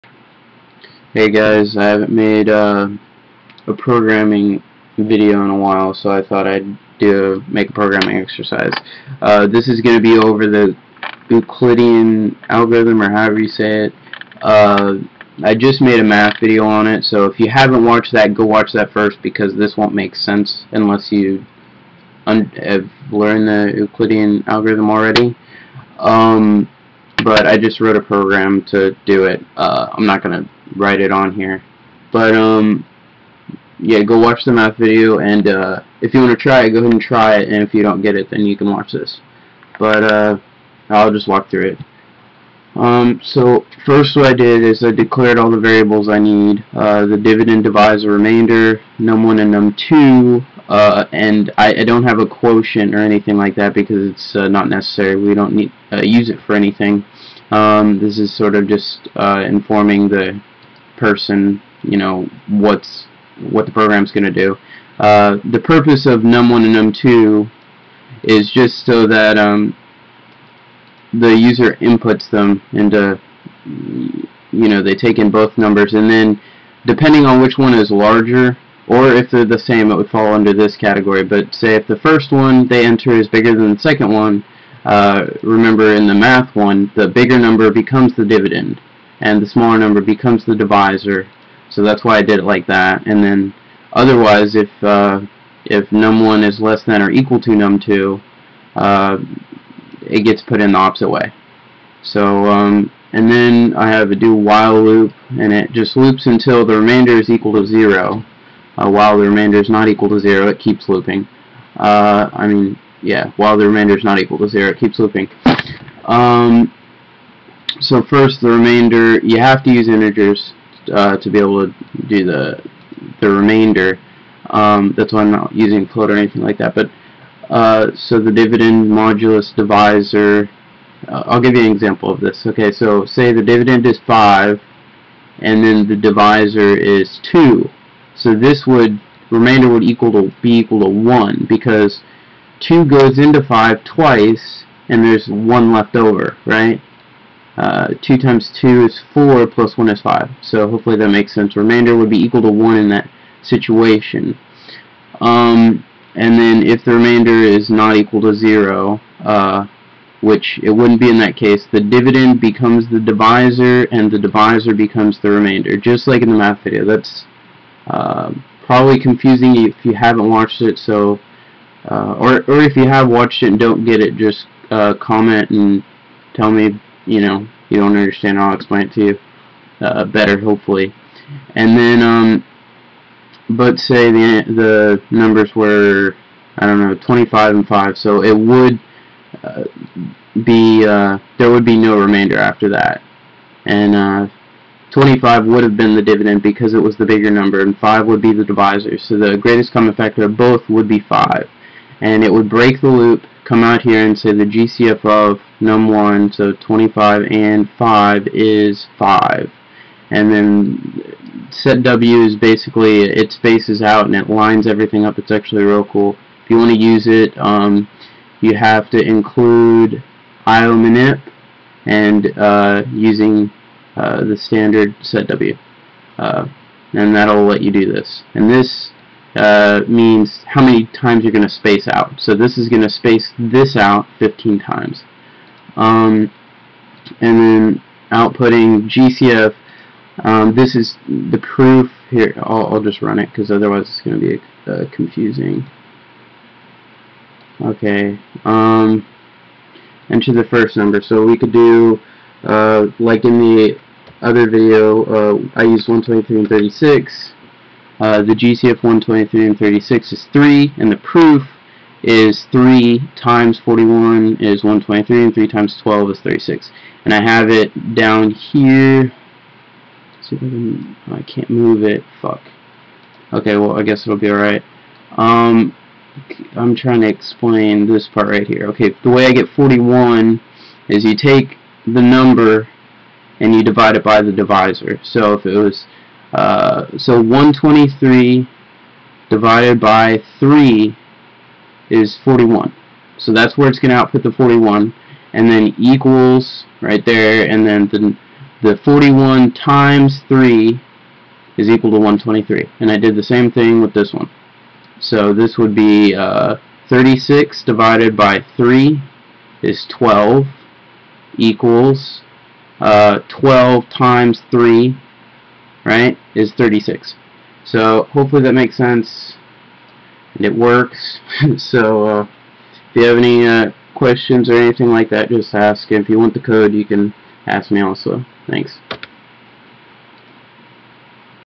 C++ Exercise 11 - Euclidean Algorithm for finding the GCF
I show how to write a program to find the Greatest Common Factor using the Euclidean Algorithm.  If you haven't watched my math video on the Euclidean Algorithm yet I highly recommend watching it.